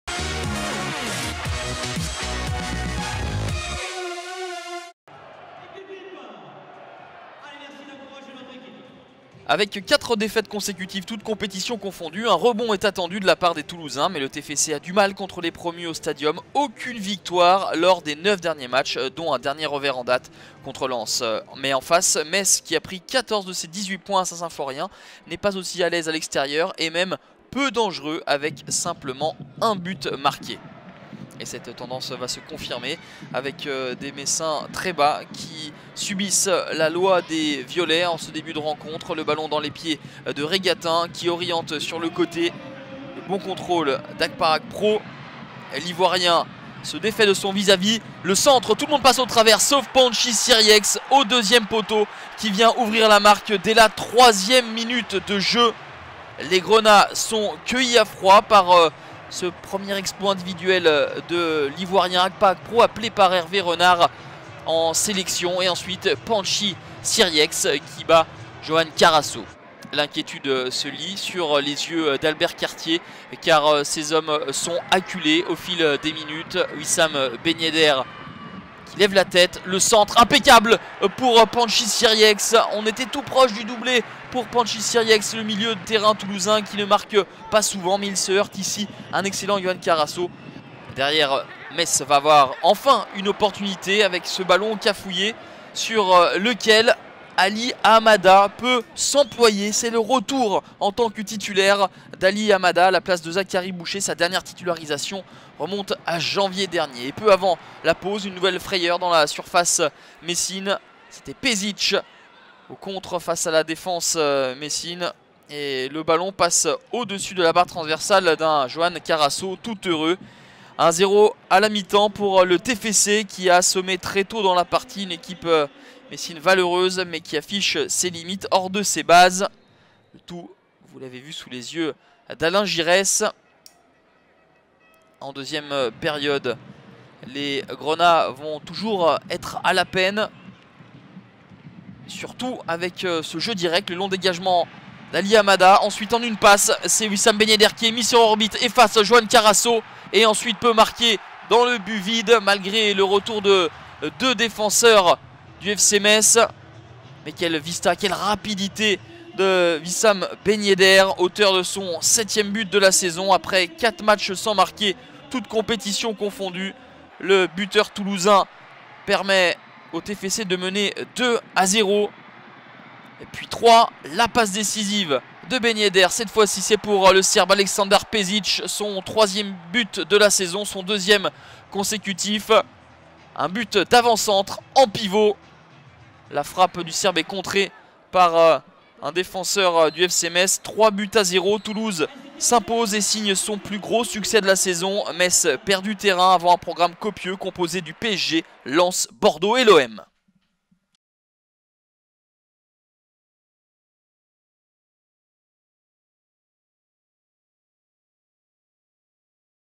Toulouse FC - FC Metz (3-0) - Résumé - (TFC - FCM) / 2014-15
Revivez les meilleurs moments de Toulouse FC - FC Metz (3-0) en vidéo.

Stadium - samedi 8 novembre 2014

Buteurs : Pantxi SIRIEIX (3' - Toulouse FC) / Wissam BEN YEDDER (61' - Toulouse FC) / Aleksandar PESIC (71' - Toulouse FC)

Composition Toulouse FC : 4 - Tongo Hamed DOUMBIA / 7 - Jean-Daniel AKPA-AKPRO / 10 - Wissam BEN YEDDER / 11 - Aleksandar PESIC / 14 - Pantxi SIRIEIX / 15 - Uros SPAJIC / 17 - Adrien REGATTIN / 21 - Abel AGUILAR (c) / 22 - Dusan VESKOVAC / 29 - Jacques François MOUBANDJE / 30 - Ali AHAMADA

Composition FC Metz : 1 - Johann CARRASSO / 5 - Guido MILAN / 13 - Florent MALOUDA / 15 - Romain METANIRE (c) / 18 - Jérémy CHOPLIN / 19 - Thibaut VION / 20 - Modibo MAIGA / 21 - Ahmed KASHI / 22 - Kévin LEJEUNE / 25 - Guirane NDAW / 27 - José Luis PALOMINO